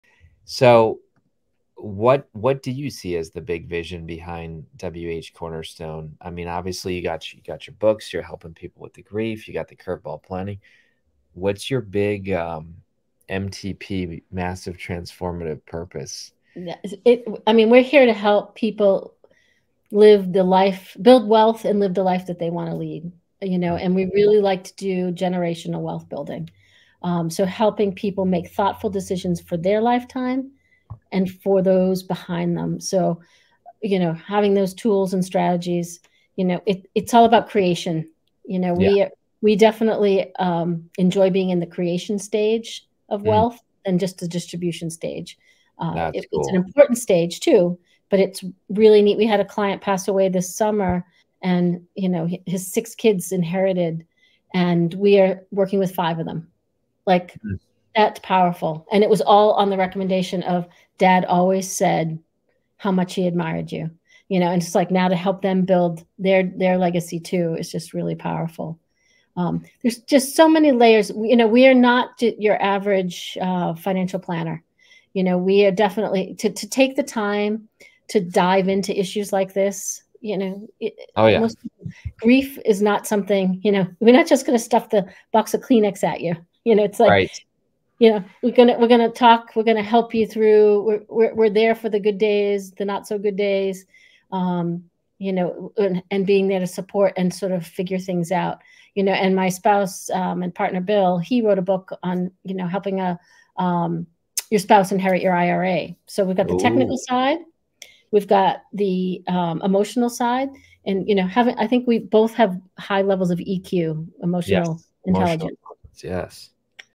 Rise Up: A Grief Journal | Livestream Launch | WH Cornerstone's Purpose
Watch this recording of Paula's livestream launch where she and Kary Oberbrunner of Igniting Souls discuss the work she and her husband, Bill Harris do at WH Cornerstone and how it serves to help families create generational wealth. WH Cornerstone's isn't your average financial planner — Paula and Bill's purpose is to help people live the life they want to lead.